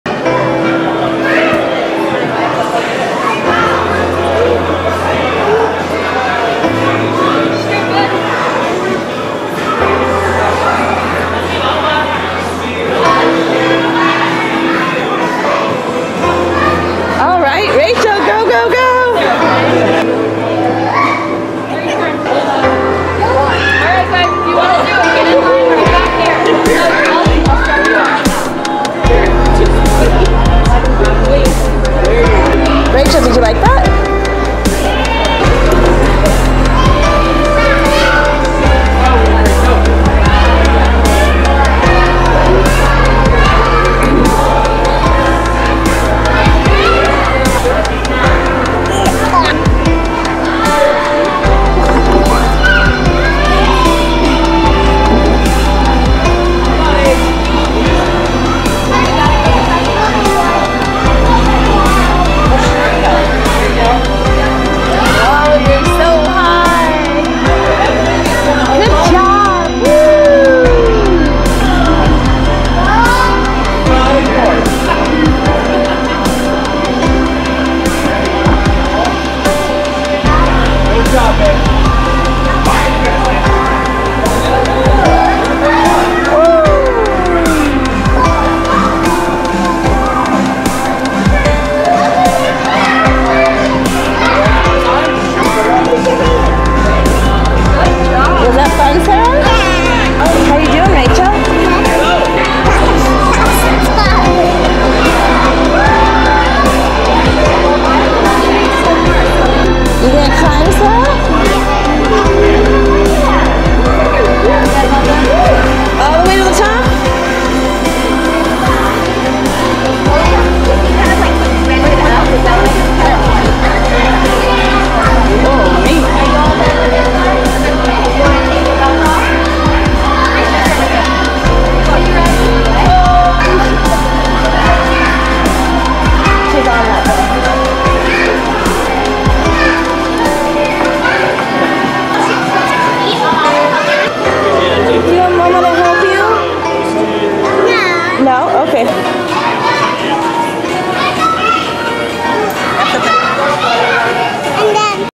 Indoor Rock Climbing Kids
Indoor Rock Climbing with kids. Sarah and Rachel attempt to climb the indoor rock climbing wall. Sarah is 5 and Rachel is 3 and you will see how far they go. What do you think?